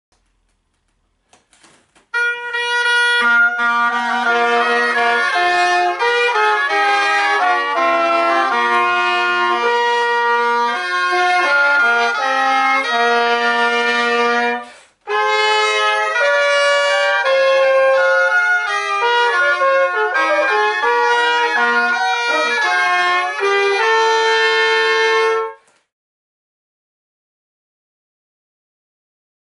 Alta Bellezza MedRen 2019
ALTA BELLEZZA

FACSIMILES, FICTA ... and trying not to FORGET
9am Saturday 6th July 2019
Kleiner Saal SCHOLA CANTORUM BASILIENSIS

Come and see us present a lecture demonstration on how and why we recreate the sound world of a 15th century Alta Capella. Why playing from facsimiles and applying historical tuning and temperament is so important and why we always play from memory.

ALTA BELLEZZA
Ann Allen SHAWMS
Hanna Geisel SHAWMS & BAGPIPE
Nathaniel Wood SLIDE TRUMPET